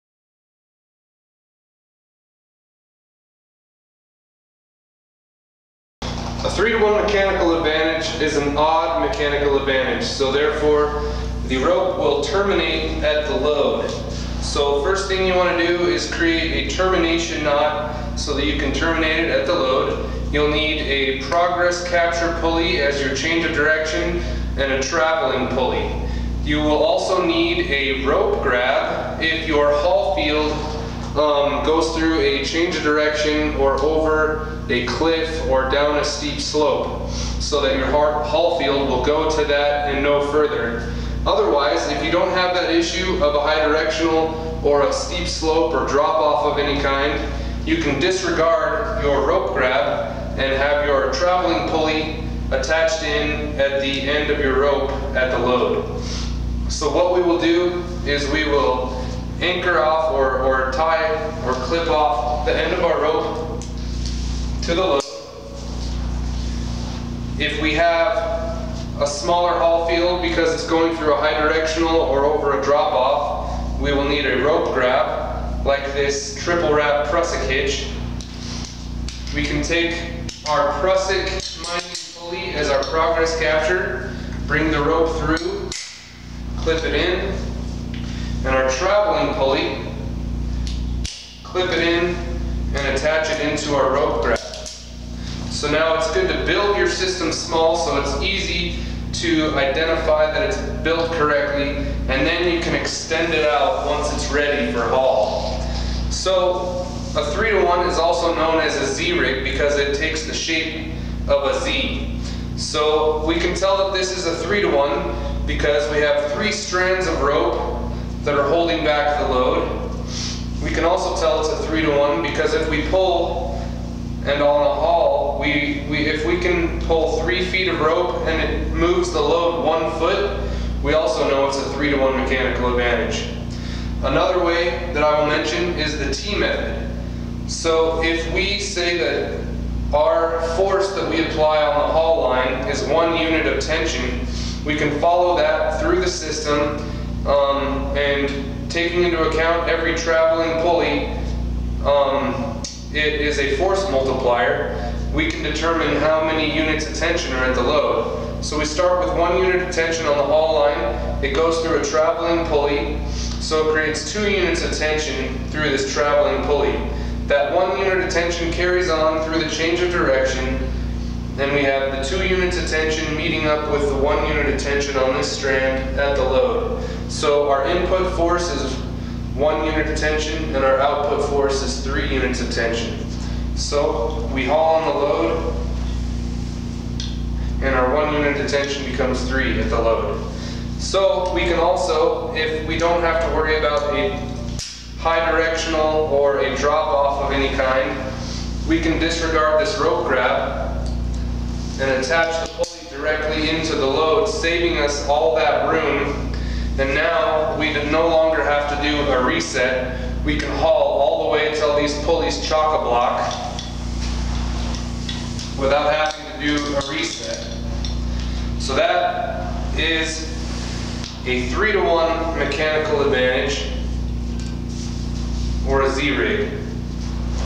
3:1 Mechanical Advantage
This video demonstrates how to build a “theoretical” 3:1.  I do not take into account the factors of friction and pulley efficiency.  If you are a physics major, this is not the video for you.  You are not my target audience